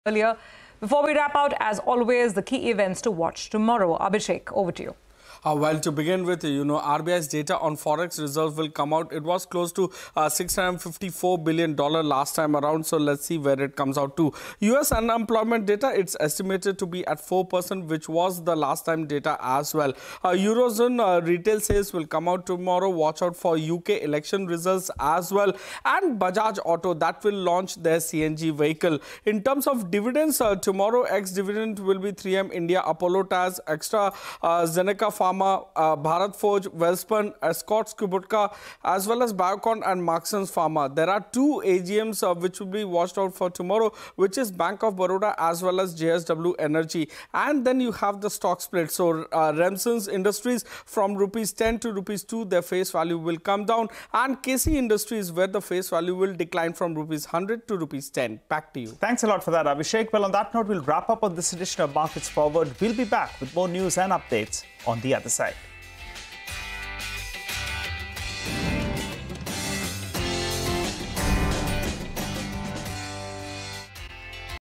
What Are Key Market Events & Cues To Watch Out For Tomorrow's Trading Session | CNBC TV18
What are the key market events to watch out for in tomorrow's session? Abhishek Kothari with more details.

n18oc_business